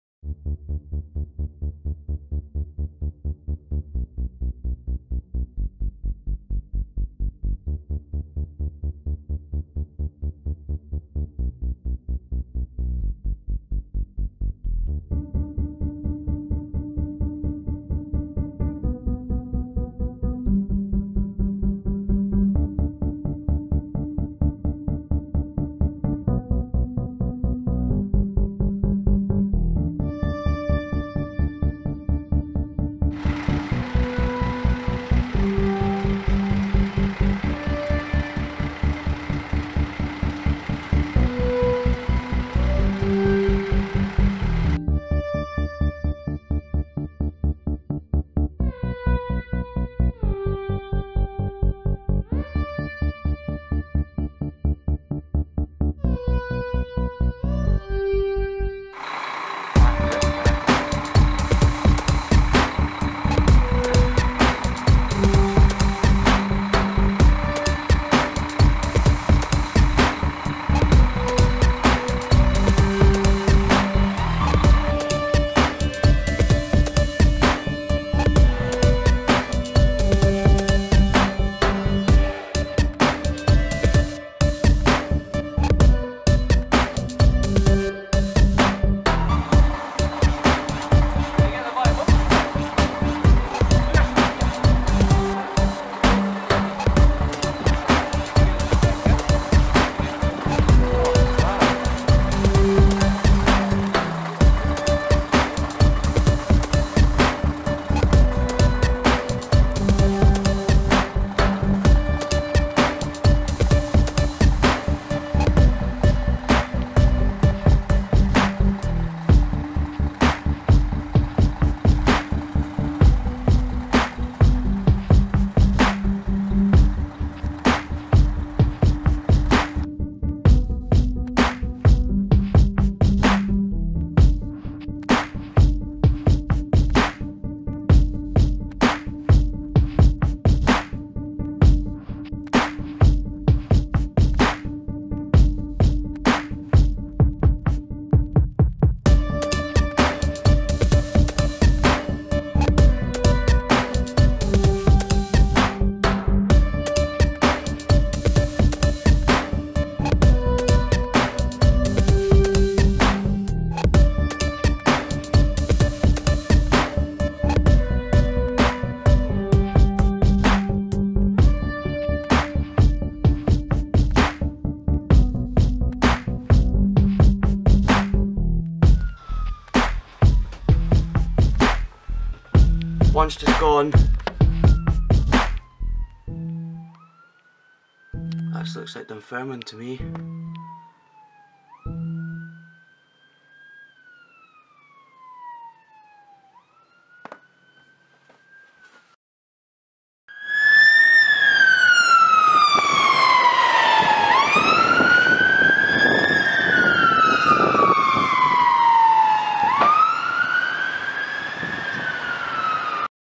Scottish Fire & Rescue Service Falkirk Fire Station
A little video i made containing photos and videos from my visit to Falkirk Fire Station and outwith.

A massive thanks is due to the firefighters at the station for allowing this to happen